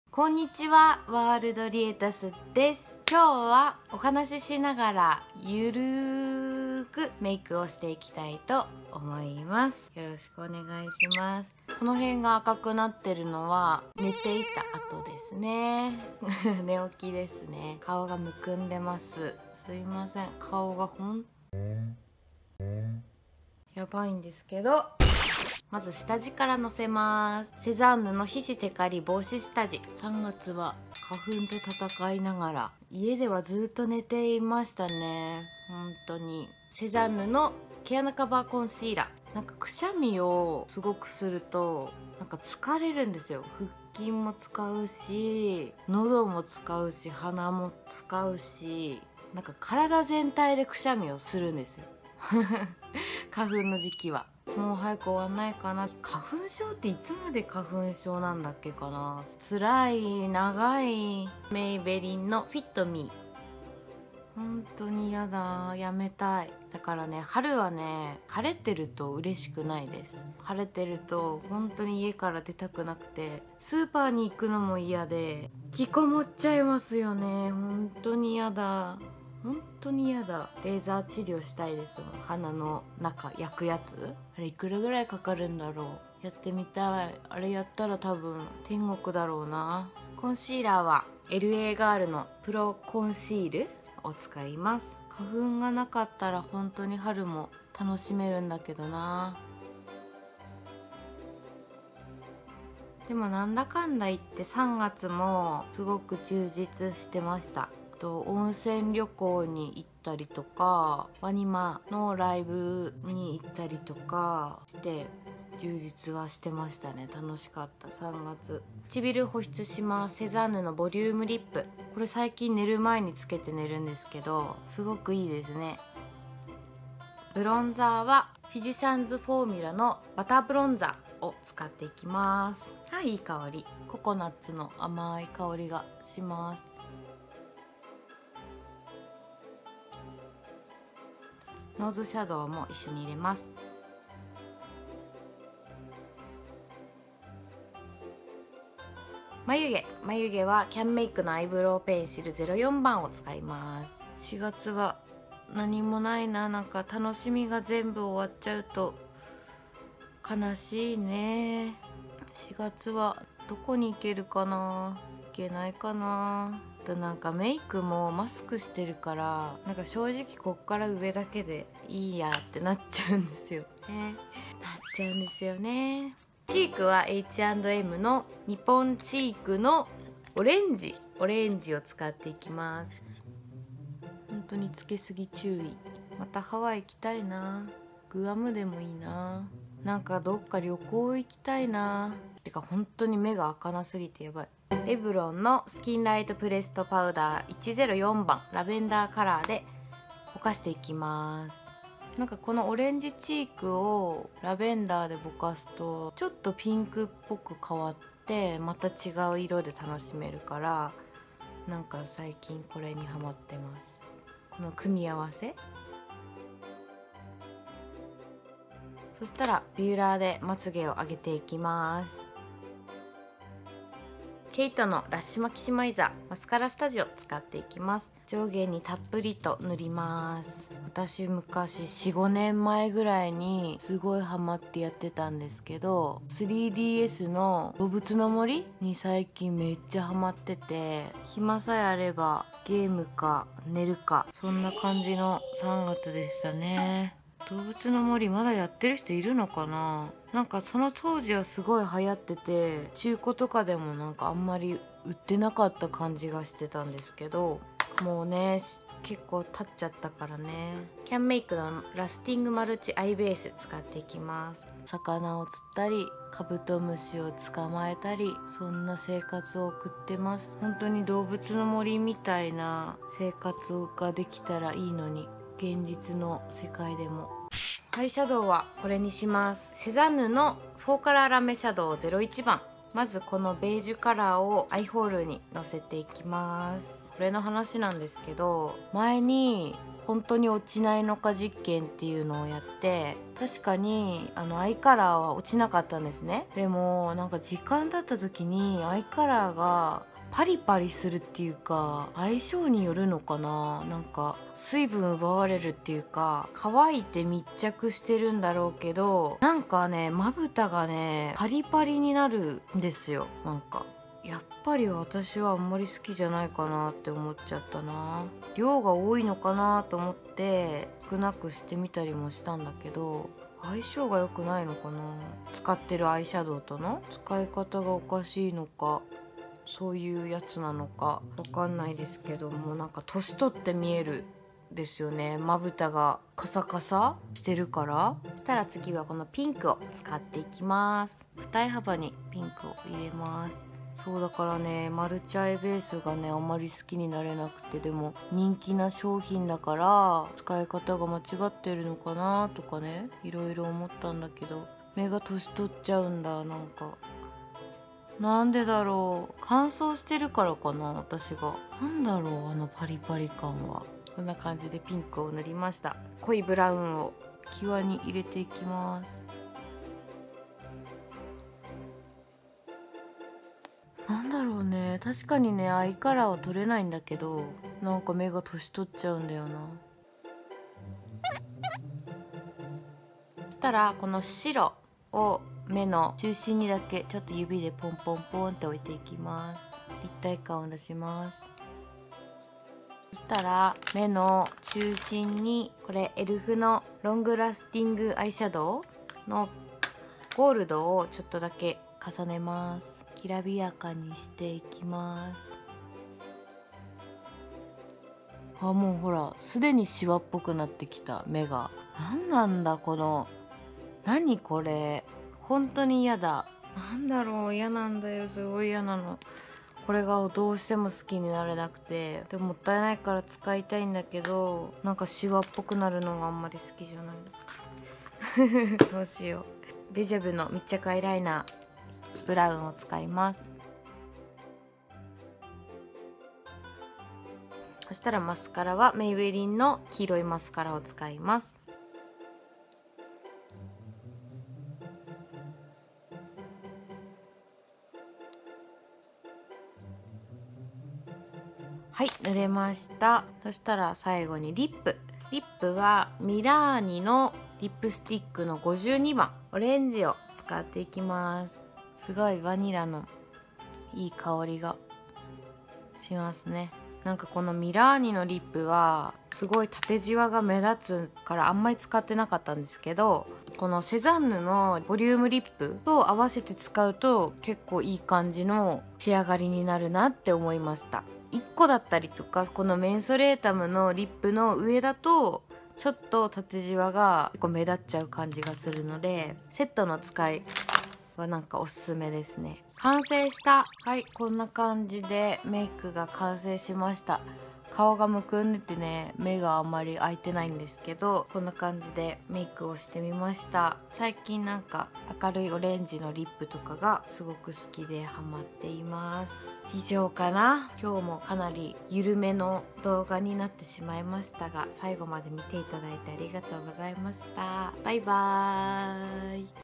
【雑談メイク】３月振り返り・辛口商品レビュー
こんにちは！わーるどりえたすです！
今回は雑談しながら春っぽメイクをしてみました。
完全にゆるーい動画です٩( ''ω'' )و